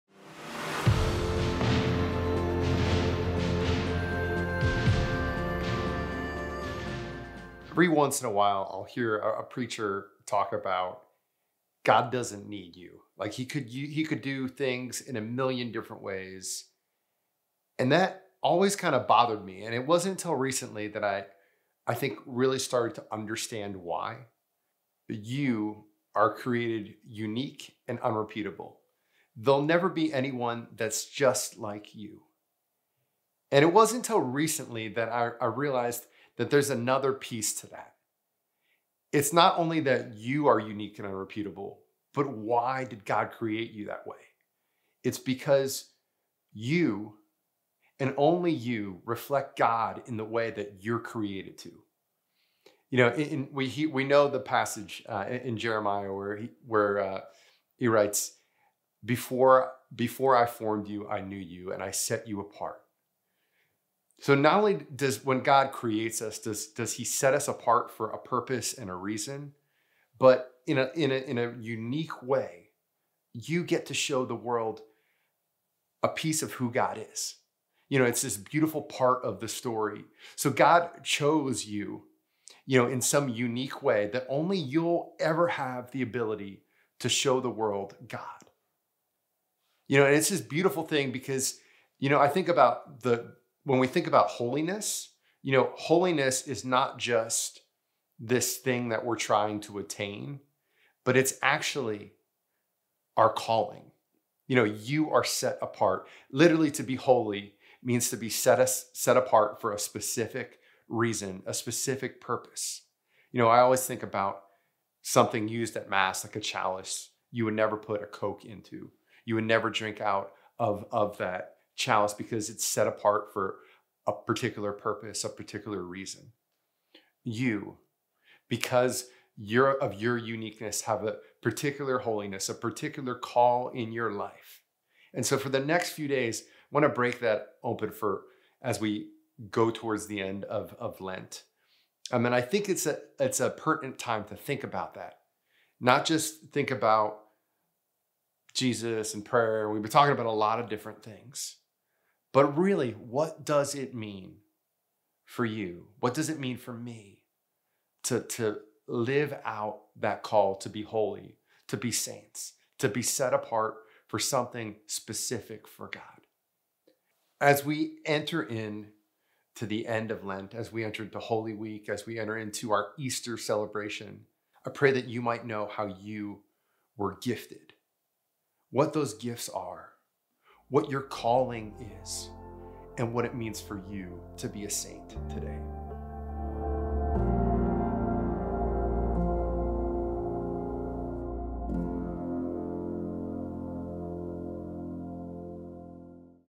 Discovering Who You Are Made to Be
“Be who you were created to be, and you will set the world on fire.” -- St. Catherine of Siena.
How do we discover who we are created to be? Andy Lesnefsky gives us some insight in today's video.